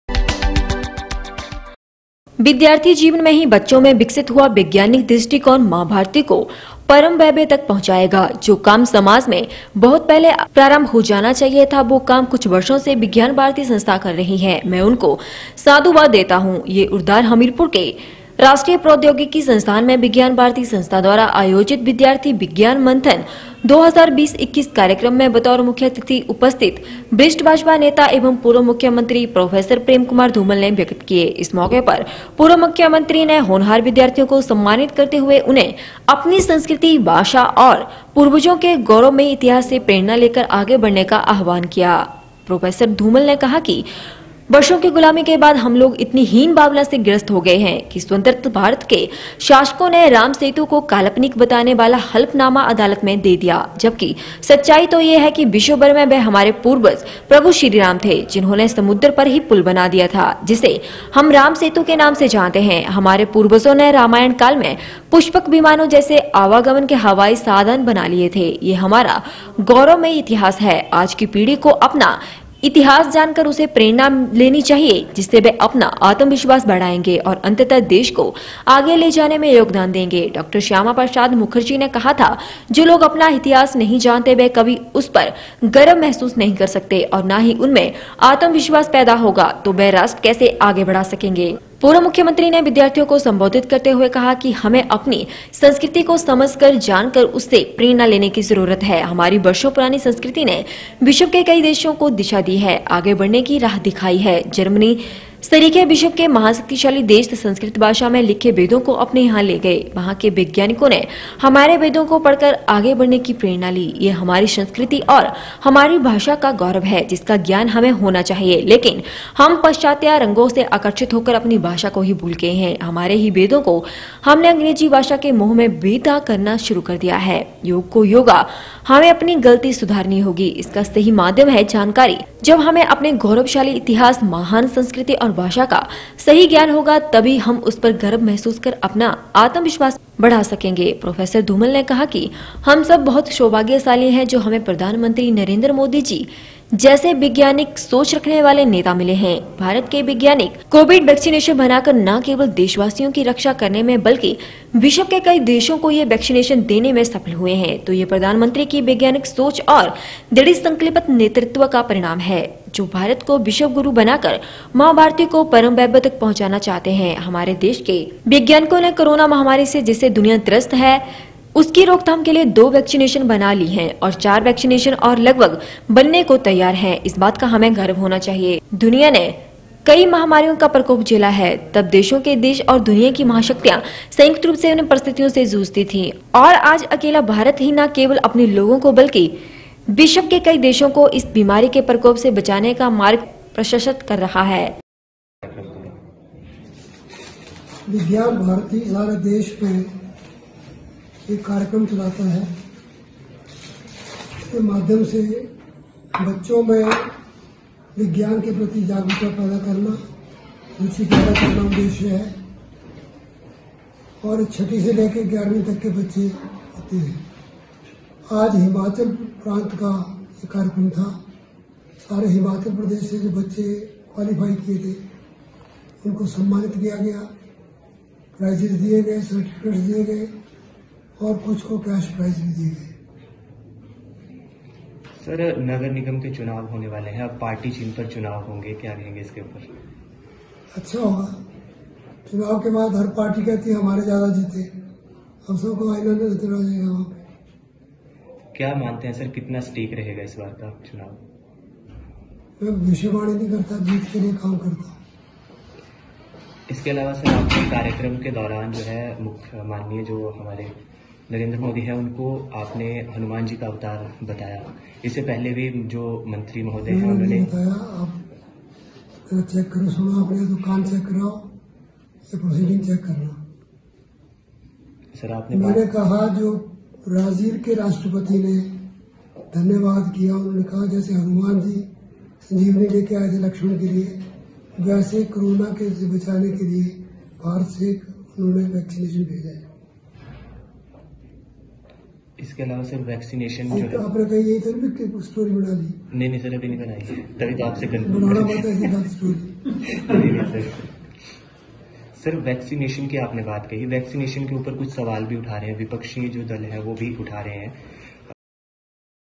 विद्यार्थी जीवन में विकसित वैज्ञानिक दृष्टिकोण माँ भारती को पहुंचाएगा परम वैभव तक : धूमल
प्रो० धूमल ने कहा कि वर्षों की गुलामी के बाद हम लोग इतनी हीन भावना से ग्रस्त हो गए कि स्वतंत्र भारत के शासकों ने राम सेतु को काल्पनिक बताने वाला हलफ़नामा अदालत में दे दिया, जबकि सच्चाई तो यह है कि विश्व भर में वह हमारे पूर्वज प्रभु श्रीराम थे जिन्होंने समुद्र पर ही पुल बना दिया था, जिसे हम राम सेतु के नाम से जानते हैं। हमारे पूर्वजों ने रामायण काल में पुष्पक विमानों जैसे आवागमन के हवाई साधन बना लिए थे